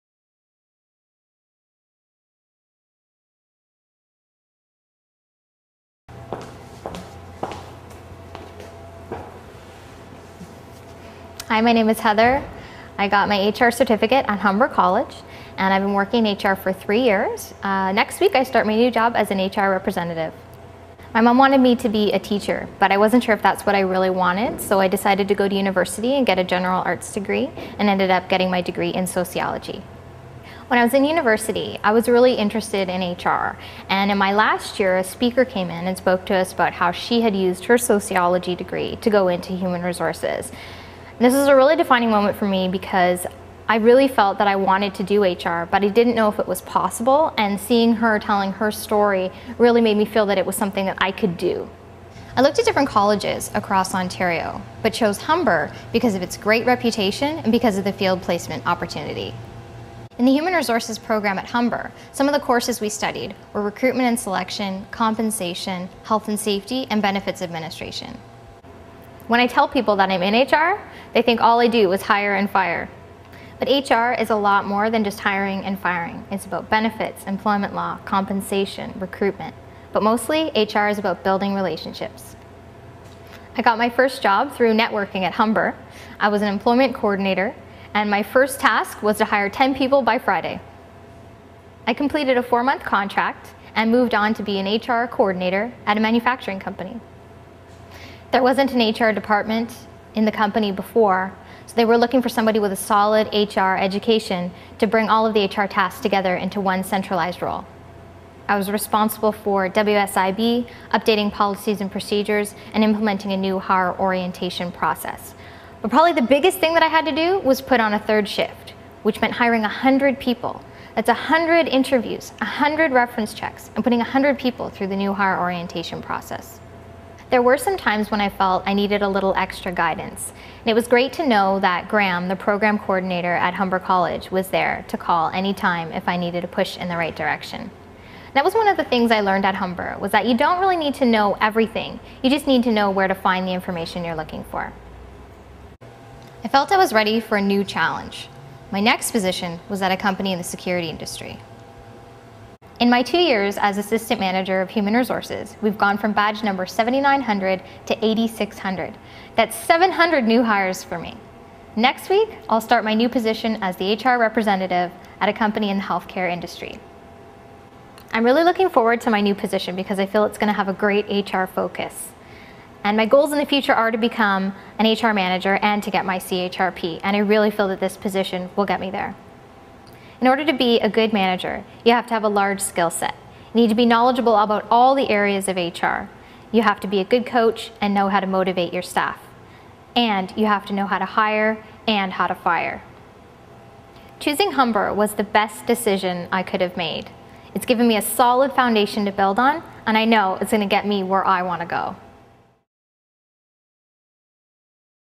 HR Management Postgrad -- Humber College
Listen to one of the graduates of the Humber College HR Management postgrad program talk about her experience and see how you could be using your experience and education to further your career in human resources.